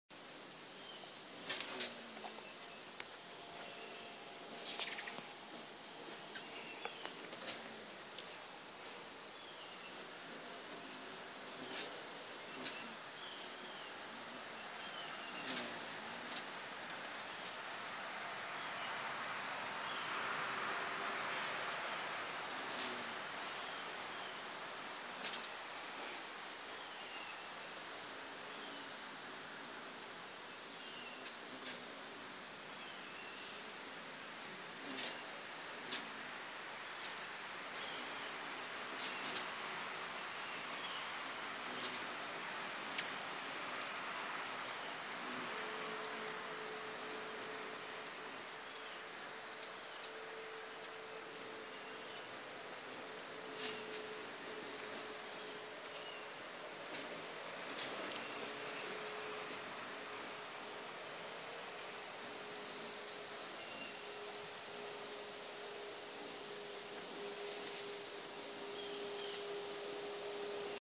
Jerk Hummingbirds
These guys have been at it for almost an hour now on the front porch.